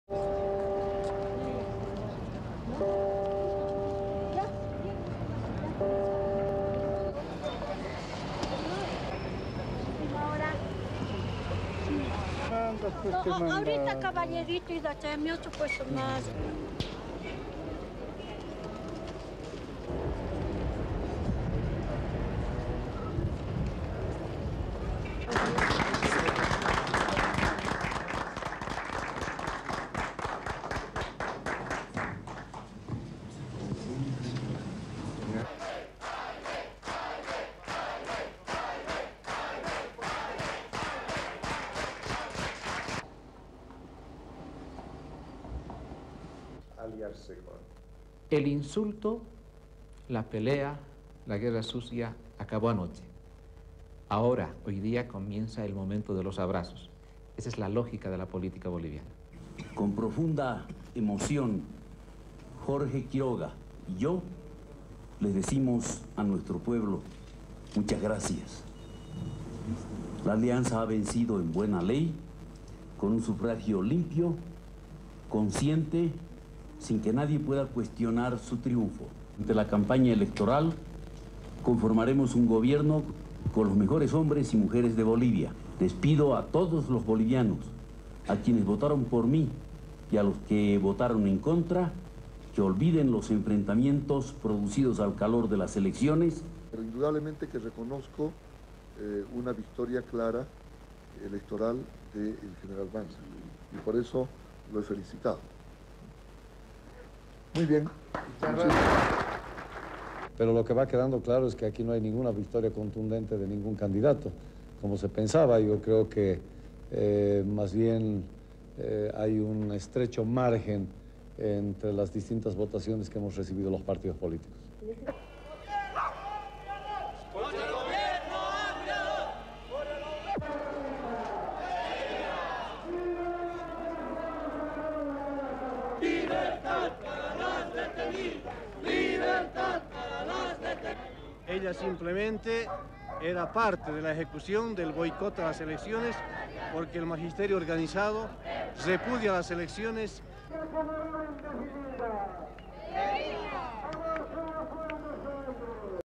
BOLIVIA: LEADING VOTE-GETTER HOLDS TALKS WITH OTHER PARTY LEADERS
(3 Jun 1997) Spanish/Nat

The leading vote-getter in Bolivia's presidential elections began talks with leaders of other parties on Monday.

Former dictator General Hugo Banzer Suarez led the field of 10 candidates in Sunday's election with 22 percent of the votes, according to unofficial results.

But because he did not received the needed majority, Congress must now choose between Banzer and the second-leading candidate, Sen. Juan Carlos Duran.

Banzer instigated the talks in the hope of gaining the support he needs to secure the presidency in an upcoming runoff.

Former military dictator Hugo Banzer was on Monday seeking support to ensure he becomes Bolivia's next president.

The retired general secured a win in Sunday's elections but failed to secure a majority.

Congress must now choose between him or the second placed candidate, senator Juan Carlos Duran, to be the country's next leader.

The decision will be made at the start of the new congressional session on 4 August.

But already, analysts say a very different political climate has set in, in the wake of a fierce and often vicious bout of political campaigning.

SOUNDBITE: (Spanish)
"The insults, the fight, the dirty war ended yesterday. Now begins the time for embracing. This is the logic of the Bolivian politics."
SUPER CAPTION: Carlos Torranzo, political analyst

Banzer ruled Bolivia with an iron hand between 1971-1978.

He now proclaims himself a sworn democrat and insists that abuses blamed on his seven year dictatorial regime in the 1970s were blown out of proportion.

On Monday, despite his incomplete victory, Banzer was in a mood for celebration.

SOUNDBITE: (Spanish)
"With profound emotion Jorge Quiroga (his vice-oresidential mate) and I say to our people, many thanks. Even according to proper law, with clean and conscientious practice, no one can question its trumph. With the electoral campaign, we will form a government with the best men and women of Bolivia. I ask all Bolivians who voted for me and those who voted against, that they forget the fierce confrontation in the heat of the elections."
SUPER CAPTION: General Hugo Banzer, newly elected president

Former president Jaime Paz Zamora, who came in third according to the unofficial results, conceded defeat.

SOUNDBITE: (Spanish)
"Really, what I know is that it was a clear victory, the election of General Banzer. For this reason I congratulate him. Very good."
SUPER CAPTION: Jaime Paz Zamora, former presidential candidate

Senator Juan Carlos Duran, of the ruling Nationalist Revolutionary Movement, has yet to accept defeat.

Second placed Duran won an estimated 18 percent of the vote and is still officially a contender until Congress decides.

SOUNDBITE: (Spanish)
But what remains clear is that here there is no victory for any of the individual candidates as I thought. I think that, rather, there is a wide margin between the distinct votes that we have received for the political  parties."
SUPER CAPTION: Juan Carlos Duran, presidential candidate

Civil disturbances in the run up to the elections continued this week, with teachers protesting at pay conditions by marching and boycotting the elections

SOUNDBITE: (Spanish)
"They have simply not been a part of this, and have boycotted the elections because the organisers reject these elections."
SUPER CAPTION: Luis Soruco, Executive Secretary of Bolivia's teachers union

Only minimal variations in the voting results are expected when the official figures are announced by the election board later this week.